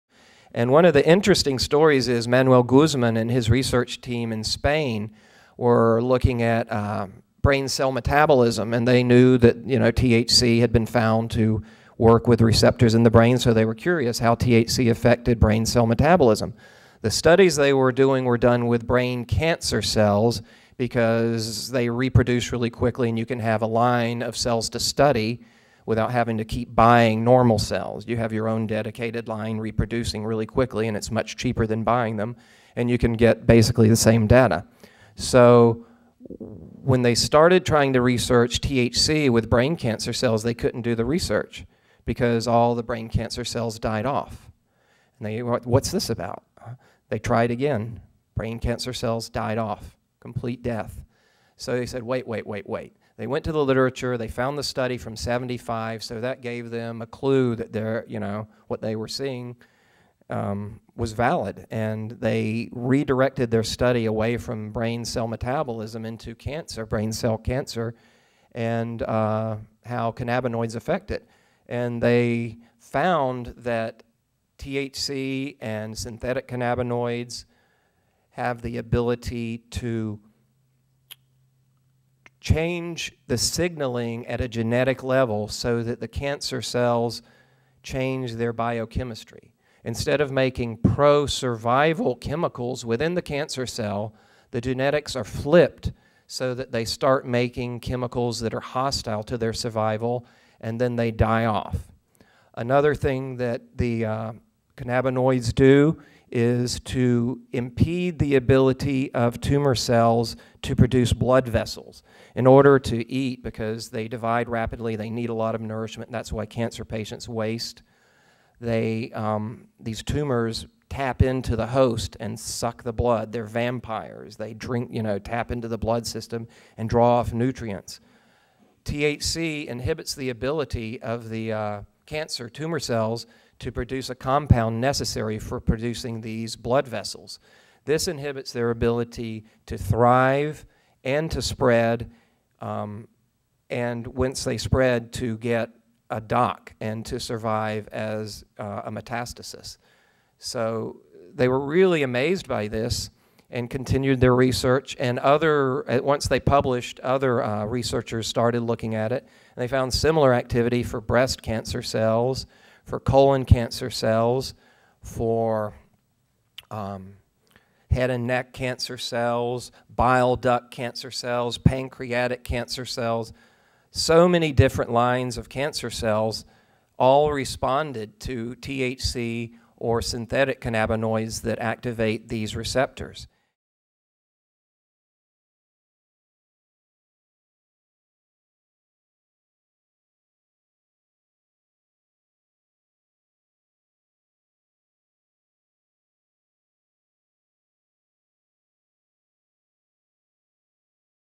How does marijuana fight brain cancer?
Clint Werner recounts the first study of THC and brain cancer cells, which led to numerous studies on other types of cancer cells. Clint's book "Marijuana Gateway to Health" is available in our online catalog